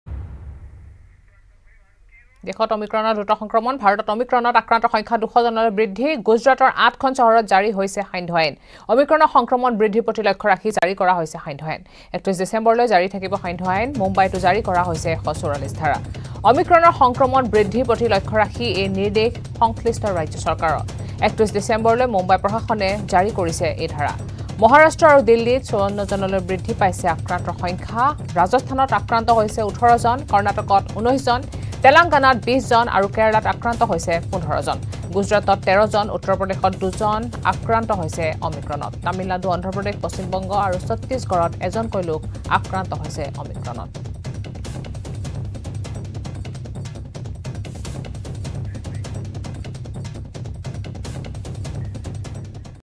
Omicron News | গুজৰাটৰ ৮খন চহৰত জাৰি সান্ধ্য আইন
21 December, 2021 : দেশত অ'মিক্ৰণৰ দ্ৰুত সংক্ৰমণ। ভাৰতত অ'মিক্ৰণত আক্ৰান্তৰ সংখ্যা ২০০ জনলৈ বৃদ্ধি।